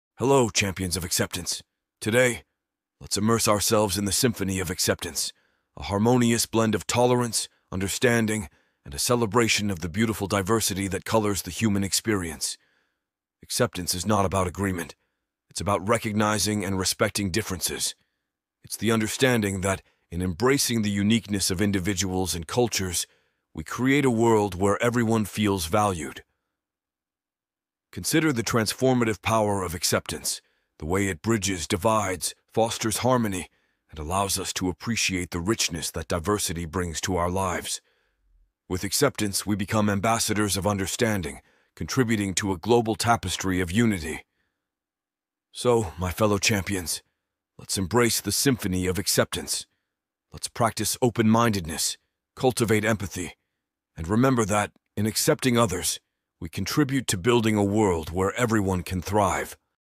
The Symphony of Acceptance / motivational video
Hello, champions of acceptance! Today, let's immerse ourselves in the symphony of acceptance—a harmonious blend of tolerance, understanding, and a celebration of the beautiful diversity that colors the human experience.
Acceptance is not about agreement; it's about recognizing and respecting differences. It's the understanding that, in embracing the uniqueness of individuals and cultures, we create a world where everyone feels valued.
Consider the transformative power of acceptance—the way it bridges divides, fosters harmony, and allows us to appreciate the richness that diversity brings to our lives. With acceptance, we become ambassadors of understanding, contributing to a global tapestry of unity.

So, my fellow champions, let's embrace the symphony of acceptance. Let's practice open-mindedness, cultivate empathy, and remember that, in accepting others, we contribute to building a world where everyone can thrive.